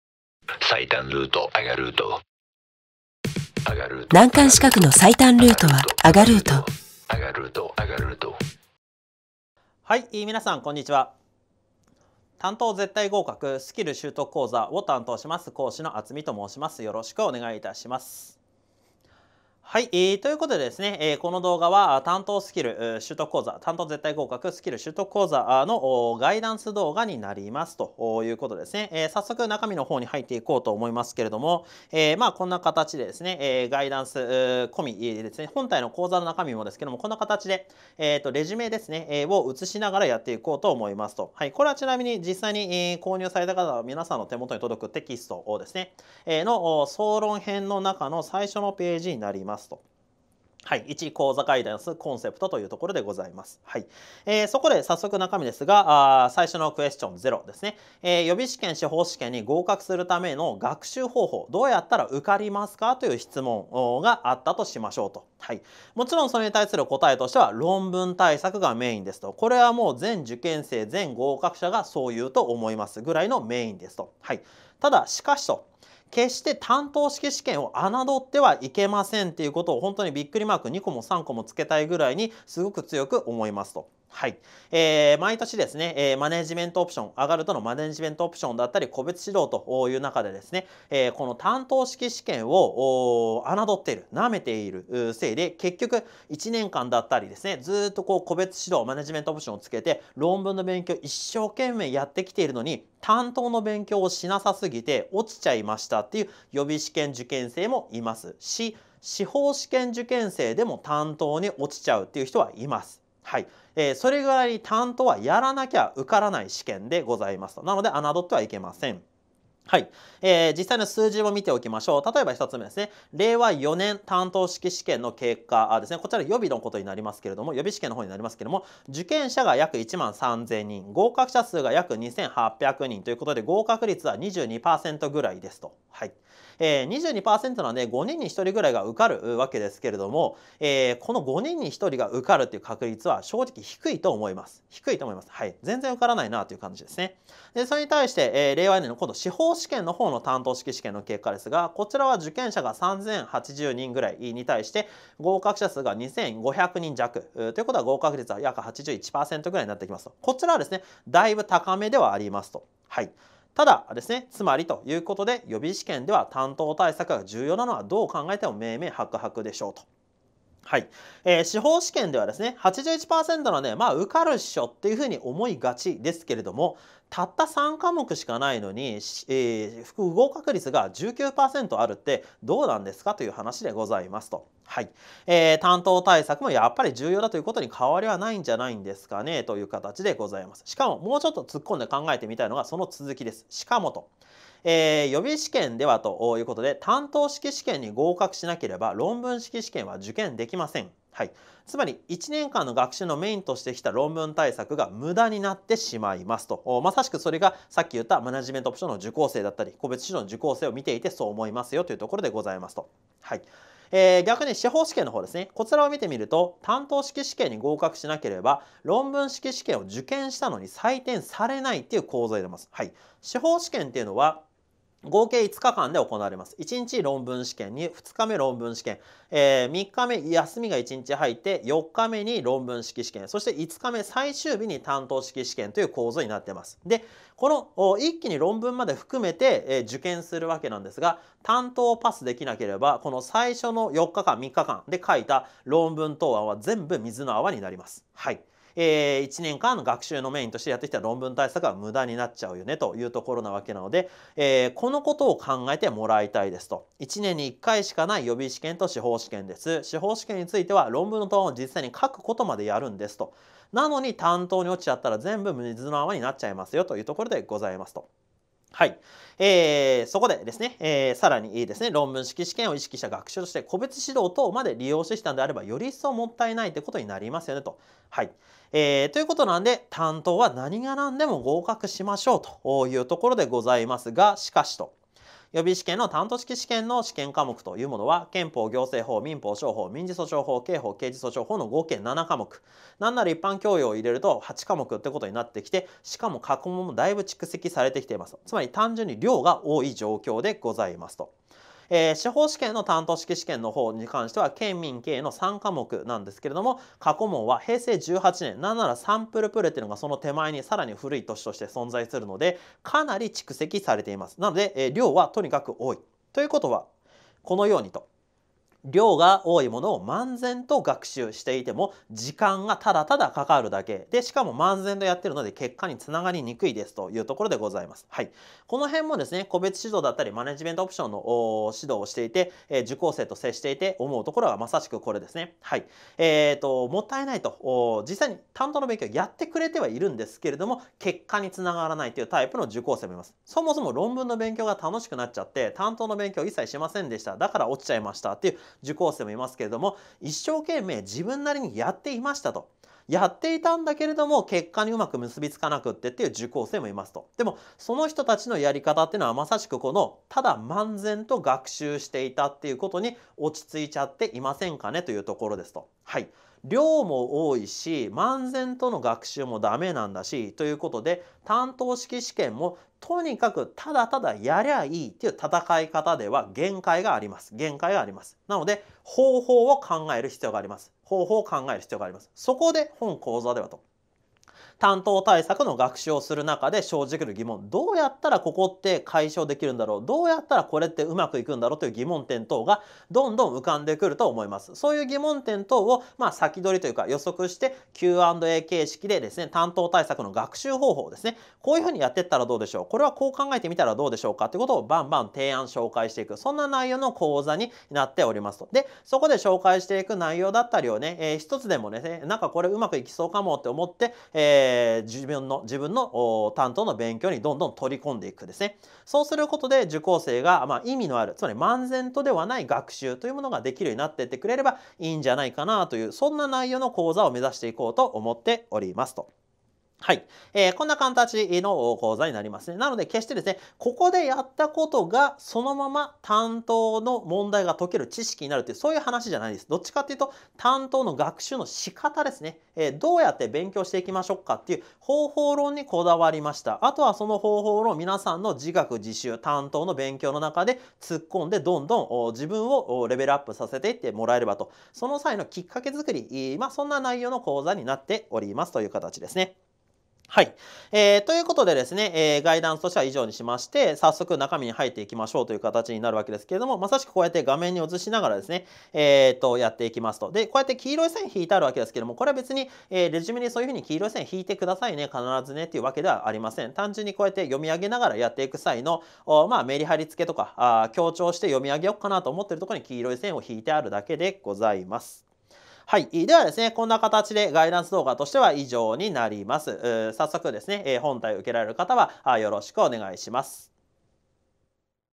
【司法試験・予備試験】短答絶対合格！スキル習得講座 ガイダンス
法律科目７科目の「短答の勉強の仕方＝スキル」を習得して、短答に絶対合格！

▼司法試験・予備試験・法科大学院入試講座

▼司法試験・予備試験・法科大学院入試のコラム

■アガルートアカデミーSNS情報
▽Twitterアカウント一覧